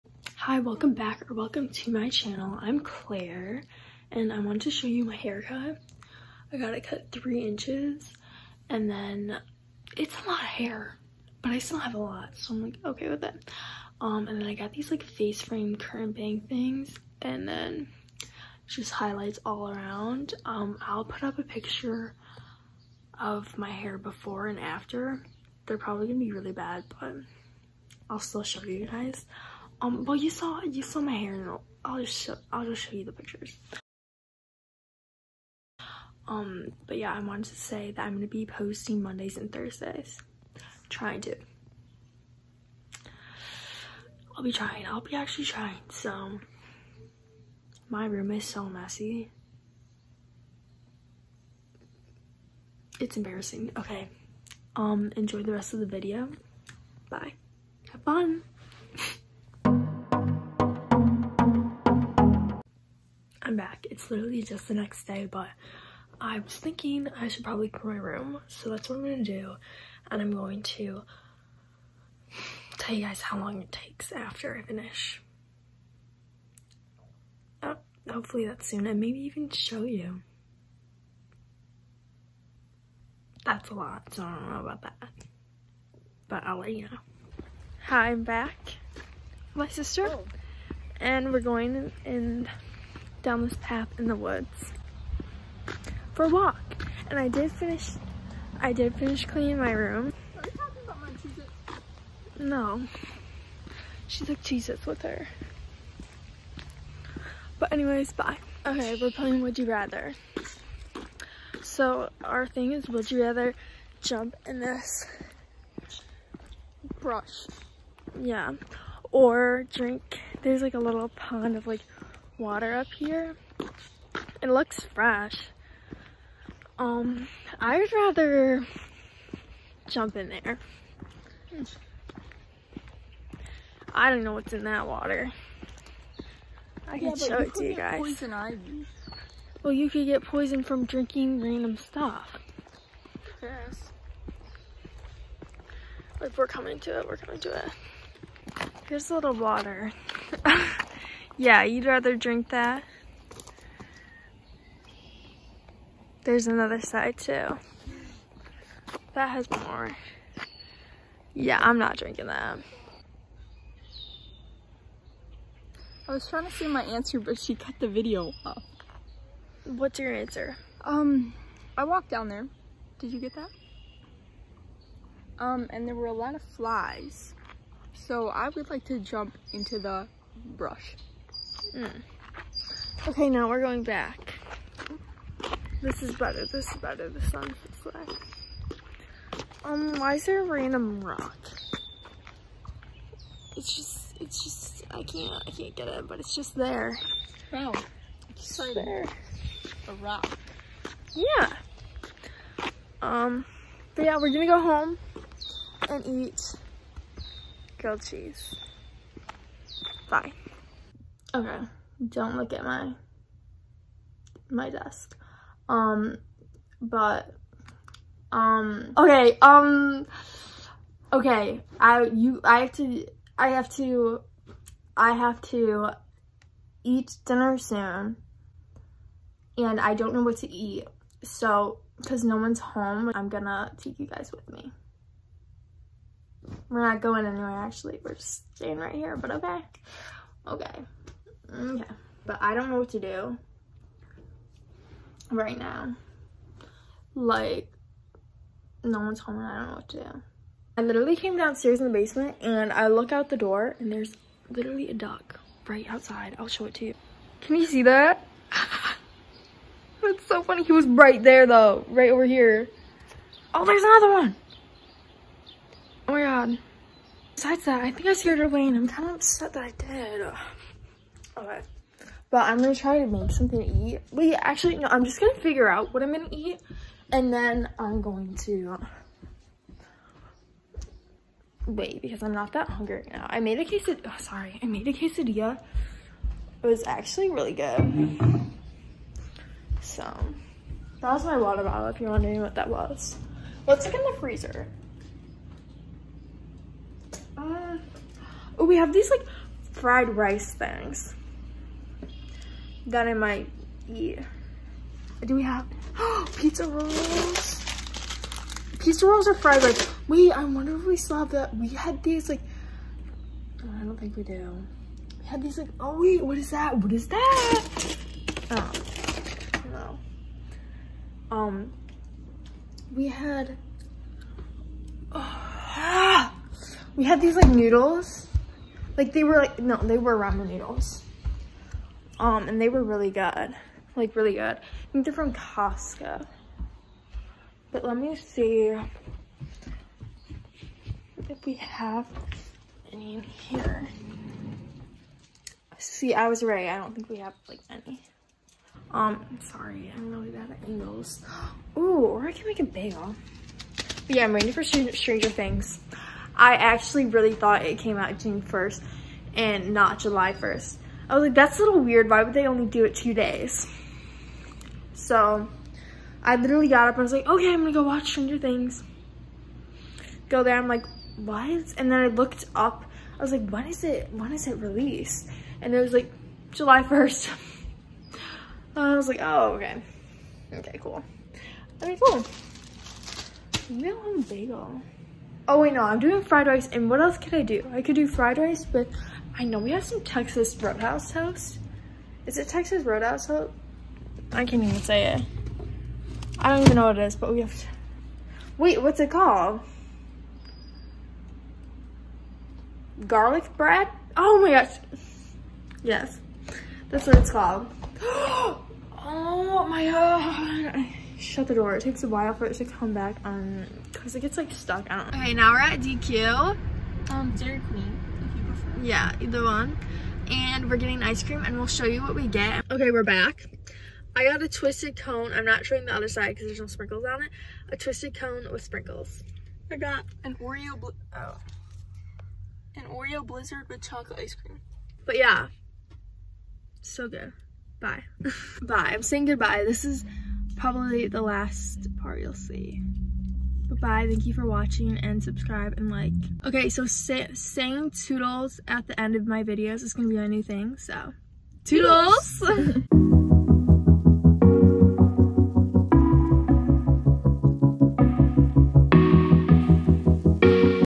Vlog :)
I (Claire) volg some days in my life. I get a haircut, clean my room, and try to make dinner. My sister (Kate) and I go for a walk and get ice cream.

Edited By:
iMovie